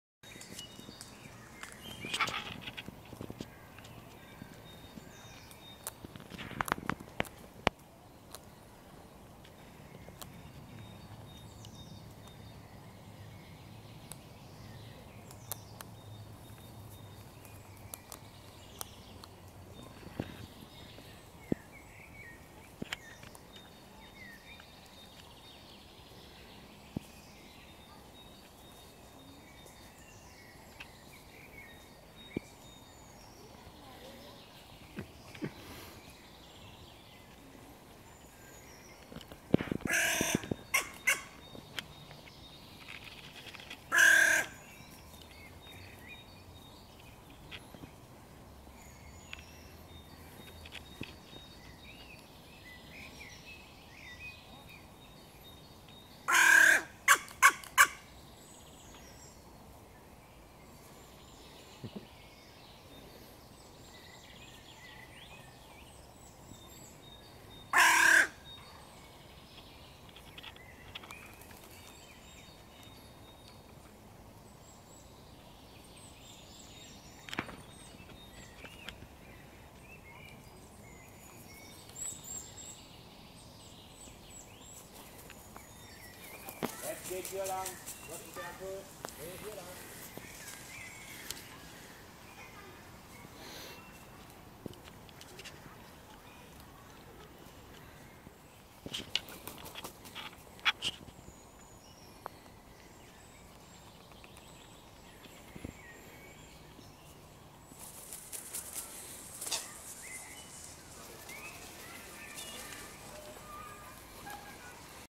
Sancho - der Kämpfer
Auch, wenn er in diesem Film etwas zurückhaltend wirkt, er hat eine bewegte Vergangenheit hinter sich. Er hat sich in einem Revierkampf oder in einer Auseinandersetzung mit einem Greifvogel oder Raubtier den Fuß verletzt. Und es sieht so aus, als ob die Fehlstellung nicht mehr zurückgeht. Aber er kümmert sich  fleißig weiter um sein Frauchen und seinen Nachwuchs. Ich hoffe,ich bekomme ihn bald zu sehen. :)